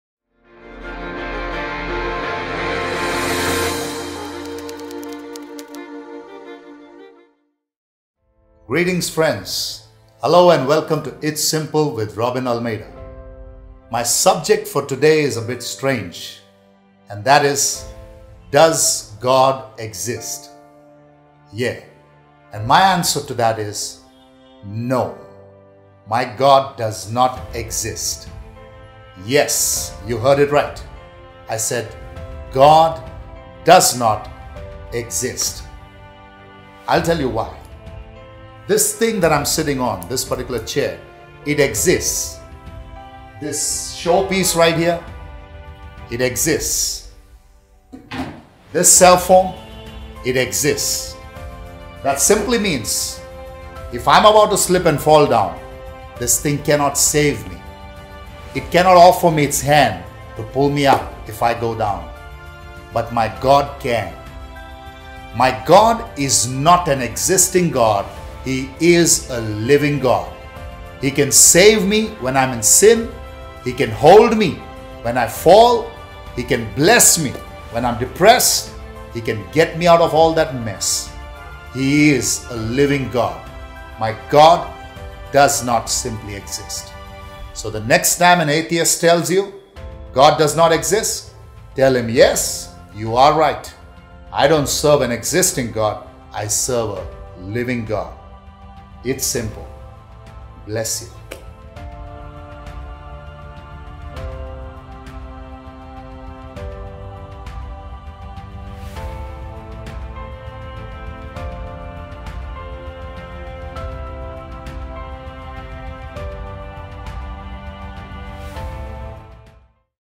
Christian Preacher Robin Almeida says God does not exist- It's Simple
Watch how Robin Almeida simplifies the most complicated mysteries of heaven in his new series - It's Simple.